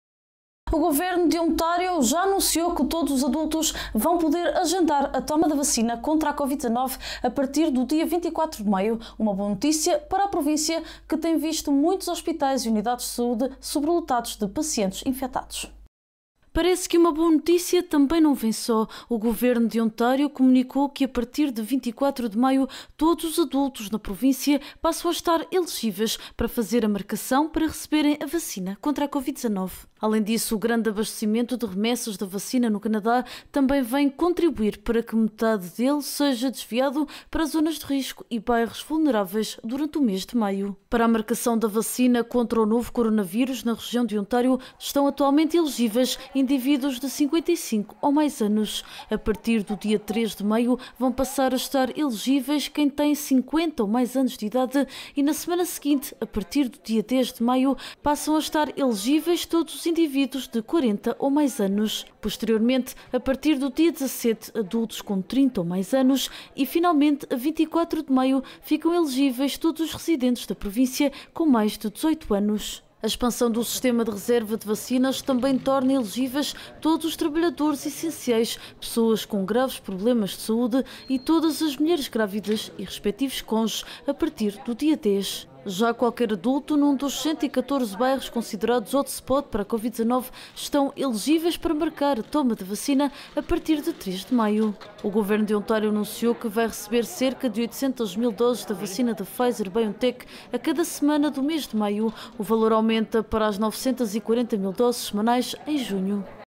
Vacinação - Adultos com mais de 18 anos, em Ontário, elegíveis a partir de 24 de maio
O Governo de Ontário já anunciou que todos os adultos vão poder agendar a toma da vacina contra a Covid-19 a partir do dia 24 de maio. Uma boa notícia para a província, que tem visto muitos hospitais e unidades de saúde sobrelotados de pacientes infetados.